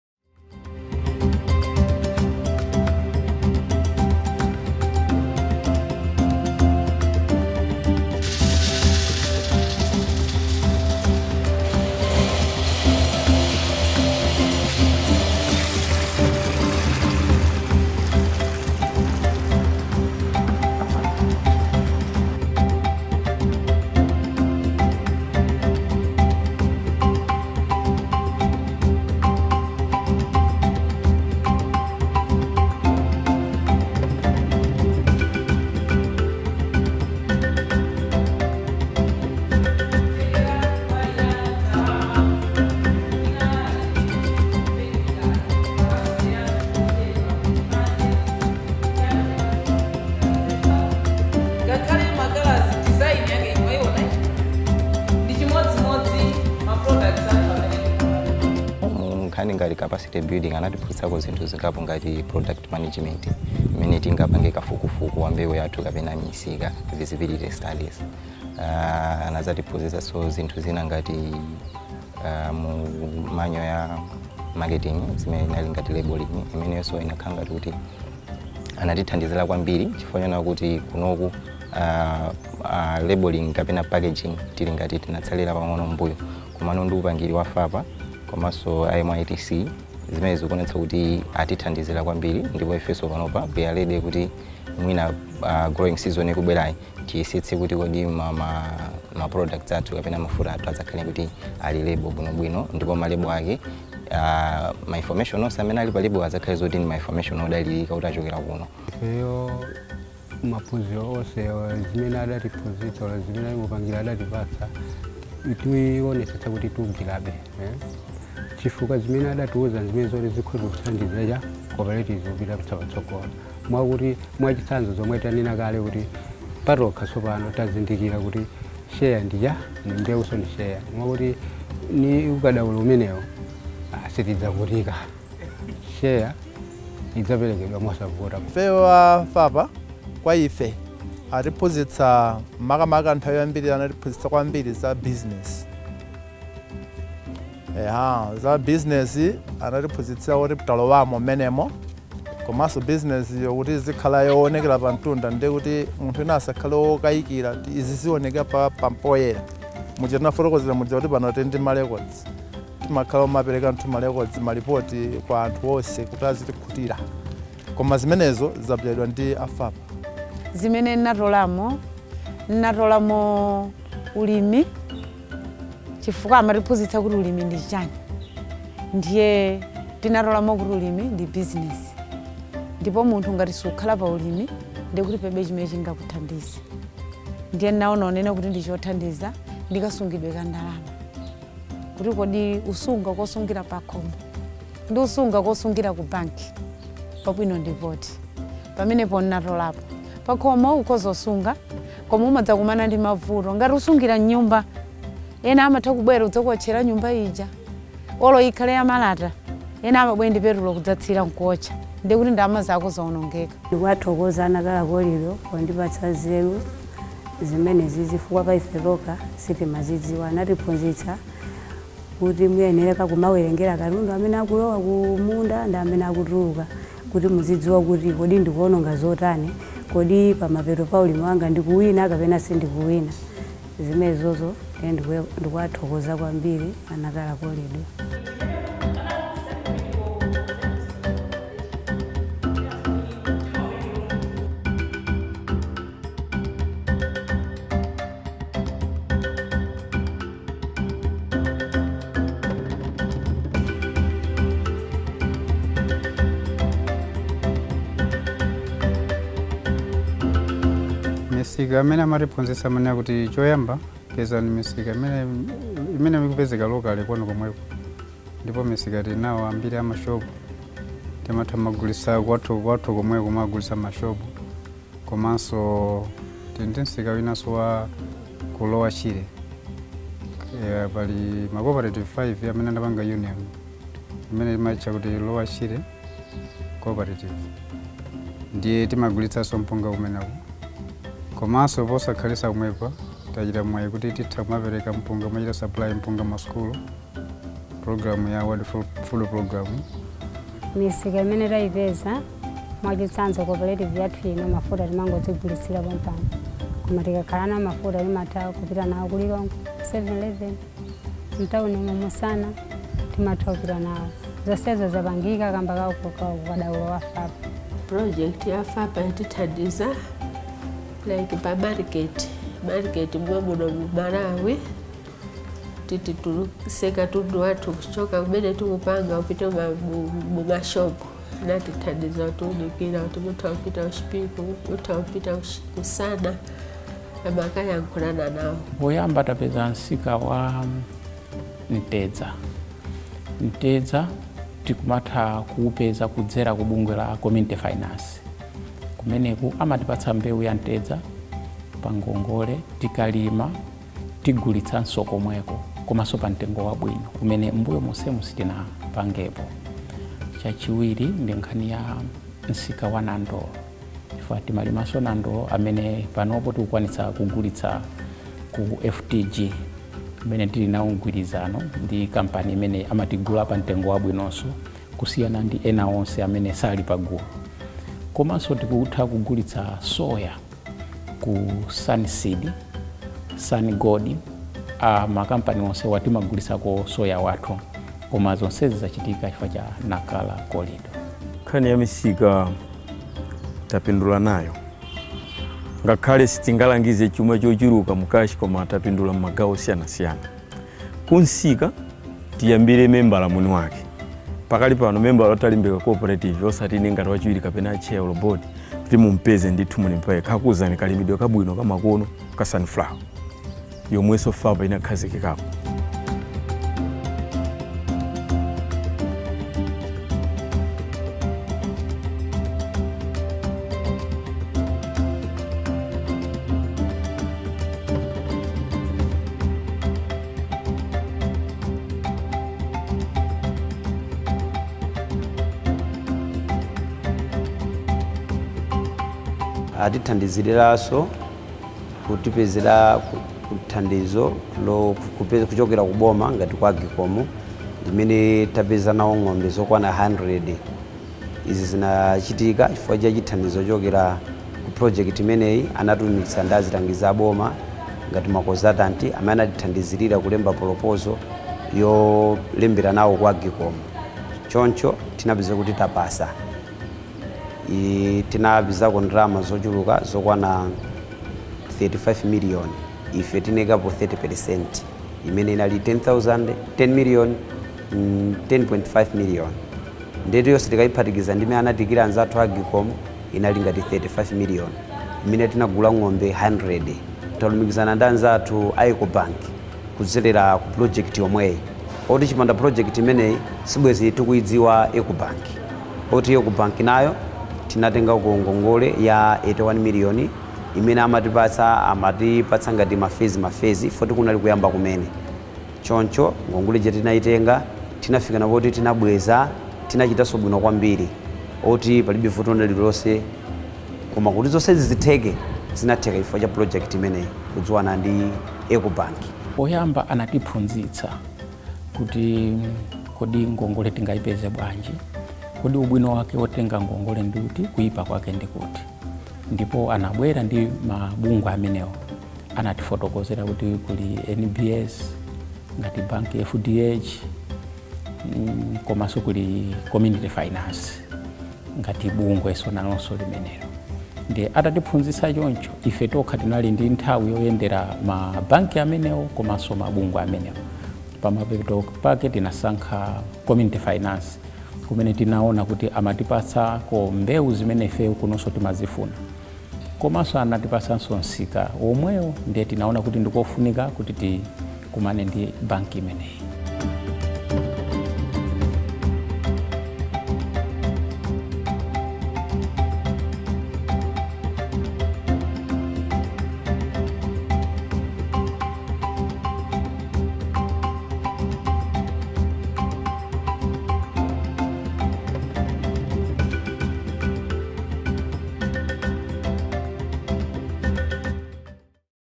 FAPA Documentary
Malawi Investment & Trade Centre (MITC) has been implementing the Nacala Rail & Port Value Addition and Inclusive Private Sector Development Project since 2017.
The project aimed at building the capacity and providing access to market and finance to growth-oriented agribusiness entrepreneurs.